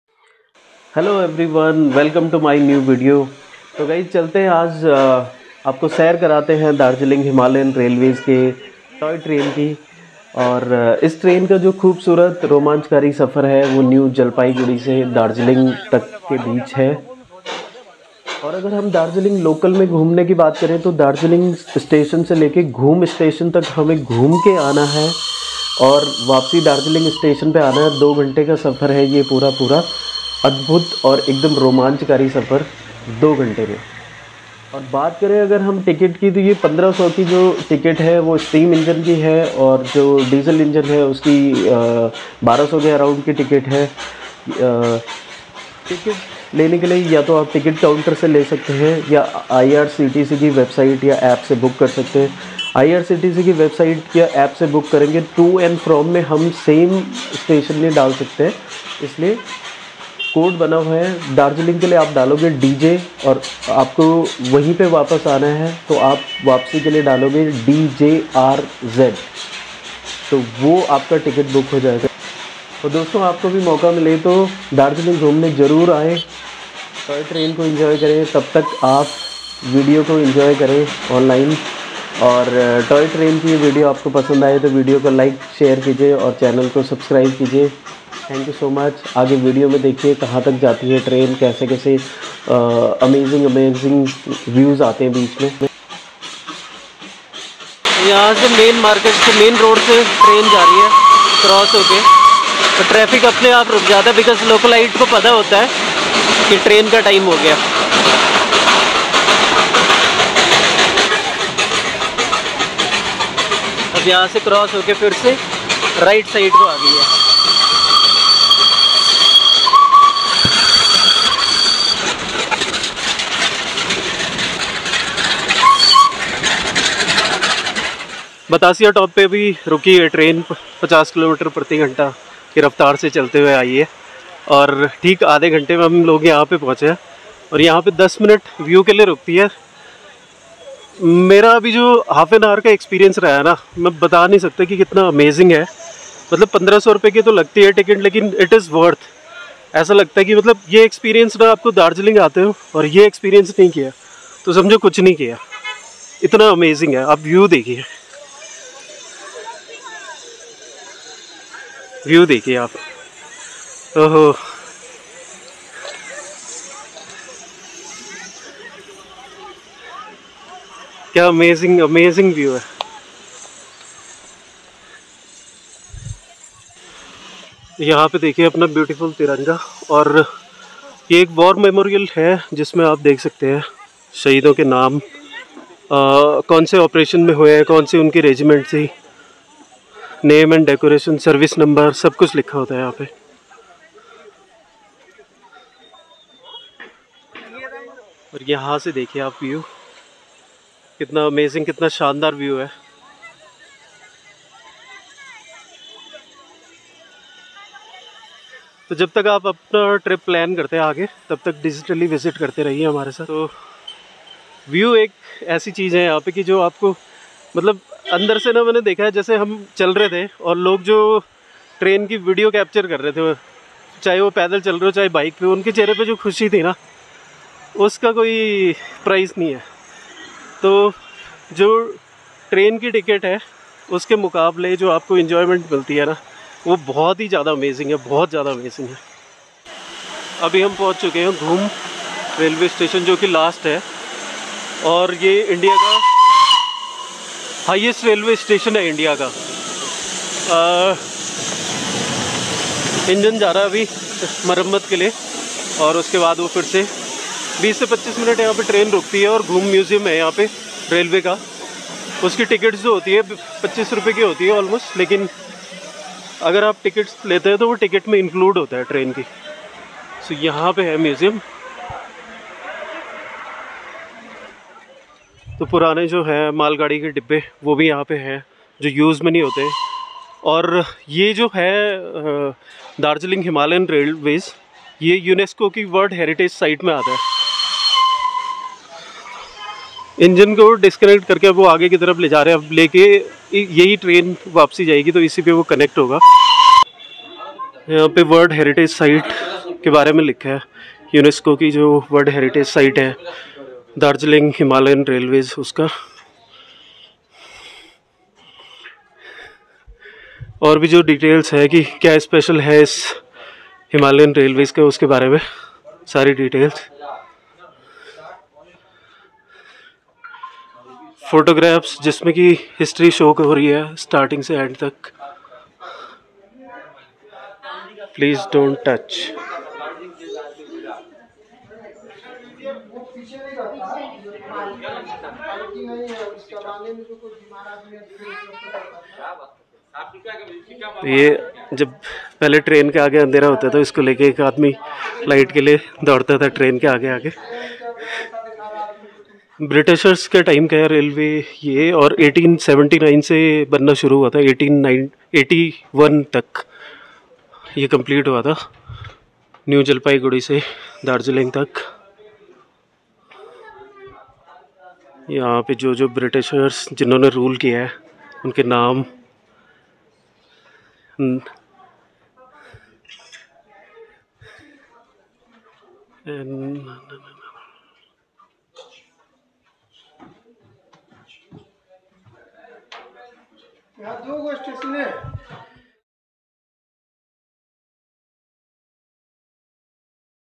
Darjeeling Toy Train Journey | Indian Railways
The beautiful toy train (joy ride) starts from Darjeeling to Ghum and back to Darjeeling takes around 2 hours. The train will stop for 10 minutes at batasia loop and 20 minutes at Ghum railway museum...

Ghum (Darjeeling Himalayan Railway) is the world's highest heritage railway station and highest railway station in India..